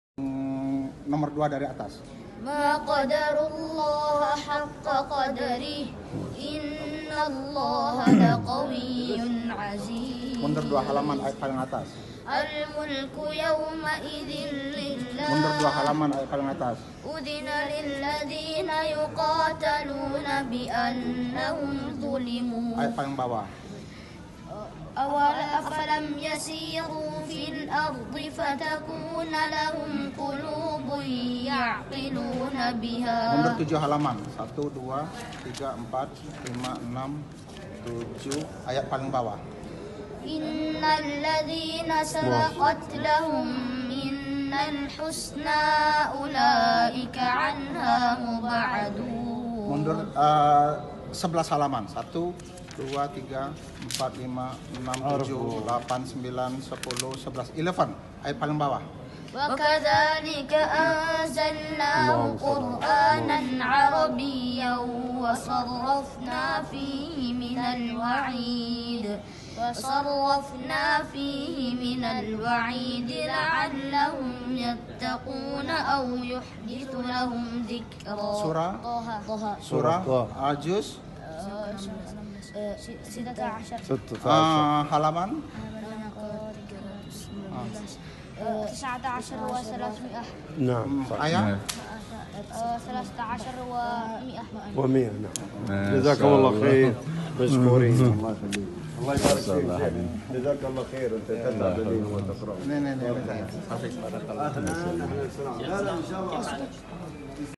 ‏طفلان إندونيسيان يتيمان هما أحمد و كامل يحفظان رقم الآية وسورتها وصفحتها والأعجب من ذلك أنك لو سألت
‏‏طفلان إندونيسيان يتيمان هما أحمد و كامل يحفظان رقم الآية وسورتها وصفحتها والأعجب من ذلك أنك لو سألته عن آية ثم قلت له ارجع قبلها ٧ صفحات مثلا واقرأ أول آية بالصفحة أو تخط ٤ صفحات واقرأ آخر آية بالصفحة يقرآن كأن المصحف مرسوم أمام أعينهما بارك الله فيهما .